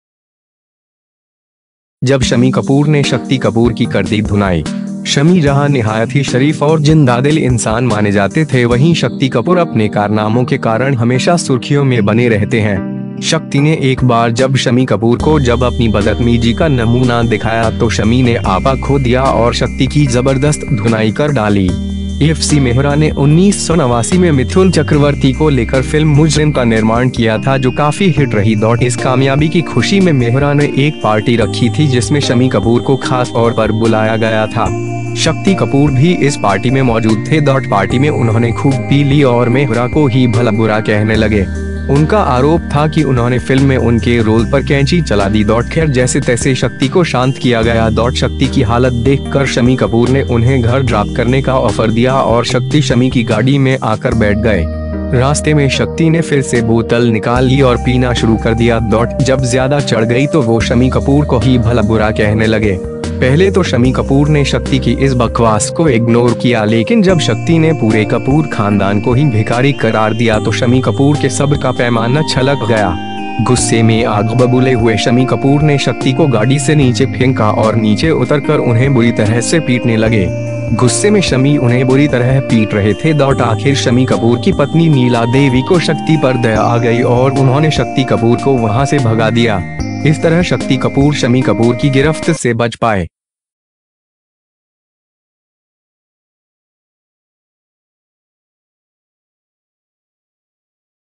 जब शम्मी कपूर ने शक्ति कपूर की कर दी धुनाई
शम्मी जहाँ निहायत ही शरीफ और जिंदादिल इन्सान माने जाते थे वहीं शक्ति कपूर अपने कारनामों के कारण हमेशा सुर्ख़ियों में बने रहते हैं. शक्ति ने एक बार जब शम्मी कपूर को जब अपनी बदतमीजी का नमूना दिखाया तो शम्मी ने आपा खो दिया और शक्ति की जबरदस्त धुनाई कर डाली .

एफ सी मेहरा ने 1989 में मिथुन चक्रवर्ती को लेकर फिल्म ‘मुजरिम का निर्माण किया था जो काफी हिट रही .इस कामयाबी की खुशी में मेहरा ने एक पार्टी रखी थी जिसमें शम्मी कपूर को ख़ास तौर पर बुलाया गया था. शक्ति कपूर भी इस पार्टी में मौजूद थे .पार्टी में उन्होंने खूब पी ली और मेहरा को ही भला-बुरा कहने लगे. उनका आरोप था कि उन्होंने फिल्म में उनके रोल पर कैंची चला दी .खैर जैसे-तैसे शक्ति को शांत किया गया .शक्ति की हालत देख कर शम्मी कपूर ने उन्हें घर ड्राप करने का ऑफर दिया और शक्ति शम्मी की गाडी में आकर बैठ गए .

रास्ते में शक्ति ने फिर से बोतल निकाल ली और पीना शुरू कर दिया .जब ज्यादा चढ़ गई तो वो शम्मी कपूर को ही भला-बुरा कहने लगे. पहले तो शम्मी कपूर ने शक्ति की इस बकवास को इग्नोर किया लेकिन जब शक्ति ने पूरे कपूर खानदान को ही भिखारी करार दिया तो शम्मी कपूर के सब्र का पैमाना छलक गया. गुस्से में आगबबूले हुए शम्मी कपूर ने शक्ति को गाडी से नीचे फेंका और नीचे उतर कर उन्हें बुरी तरह से पीटने लगे. गुस्से में शम्मी उन्हें बुरी तरह पीट रहे थे .आखिर शम्मी कपूर की पत्नी नीला देवी को शक्ति पर दया आ गयी और उन्होंने शक्ति कपूर को वहां से भगा दिया. इस तरह शक्ति कपूर शम्मी कपूर की गिरफ्त से बच पाए.